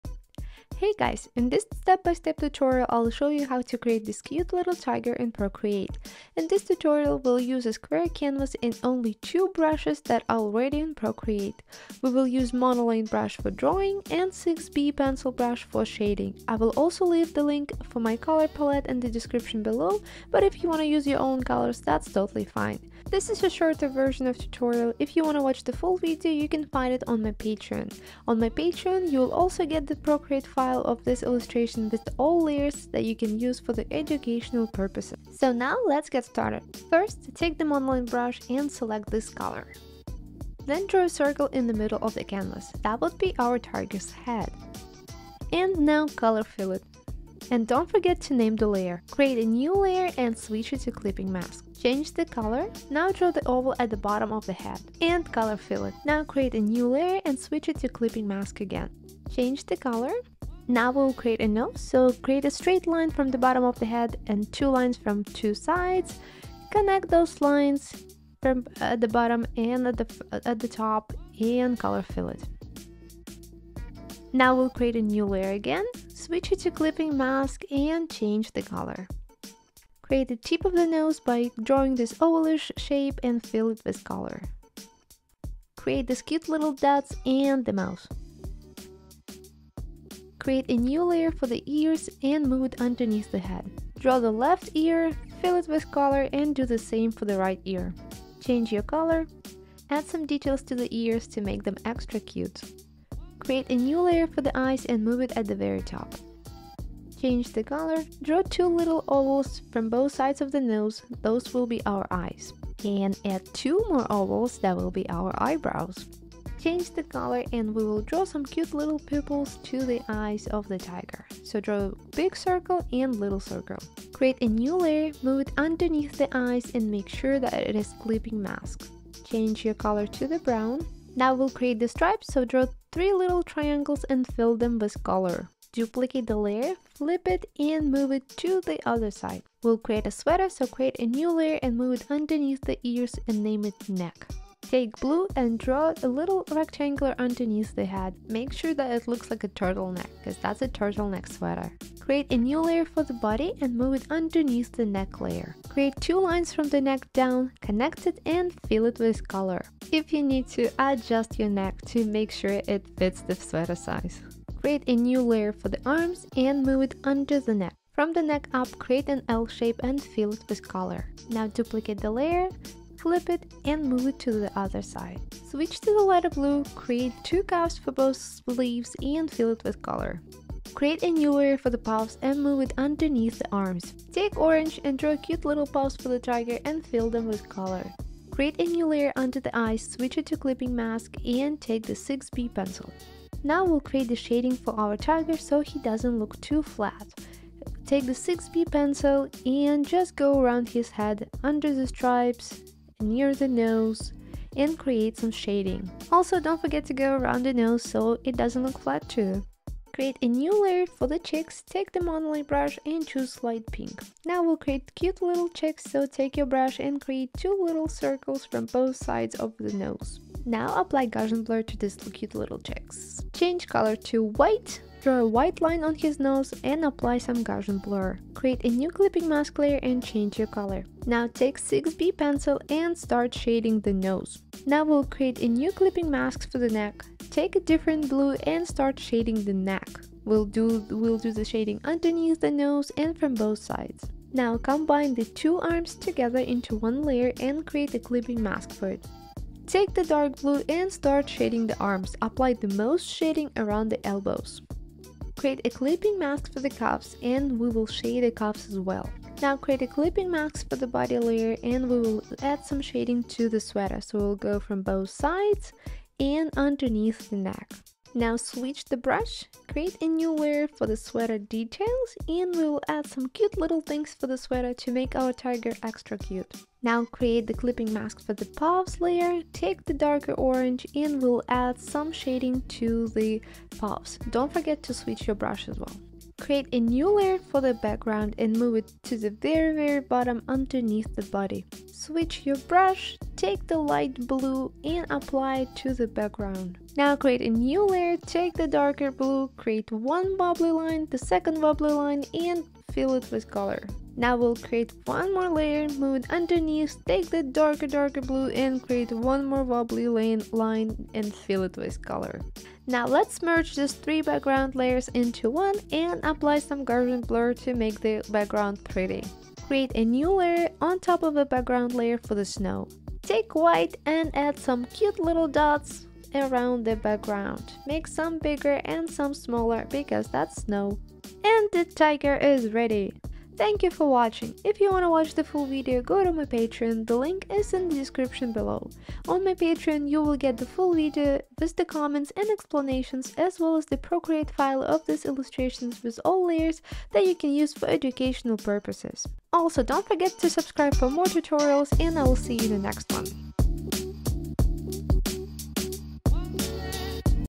Step-By-Step Drawing - Cute Tiger In Procreate on Apple iPad Pro Tutorial✏️
In this step-by-step tutorial, I will show you how to create this cute tiger in Procreate. We will use a Square 2048 x 2048 canvas and only two brushes that are already in Procreate, Monoline Brush for drawing and 6B Pencil Brush for shading.⭐️